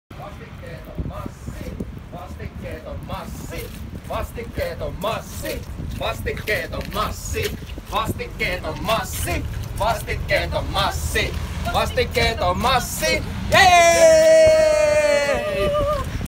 Perustulo kongajono kokeilu 2019
Olisihan se hauskaa, jos kaikillla olisi asiat hyvin. Tämän taustalle löytyy myös numeroita ja laajempia ajatuksia.
Voimme lopettaa kalliin byrokratian ja jättää ihmiselle aikaa etsiä työtä ja panostaa pienyrittäjyyteen. Kepillä ei saada mitään, ihminen ei koskaan opi uudenlaista käyttäytymistä (työhön menoa) rangaistuksen kautta. Kestävin kehitys saadaan aikaiseksi luomalla hyvinvoinnille tilaa. Perustulokokeilu on tähän mennessä näyttänyt, että hyvinvointi lisääntyi ja työllistyminen on aivan yhtä hyvää (piirun parempaakin) kuin vertailuryhmässä, joten eivät ihmiset kotiin jää.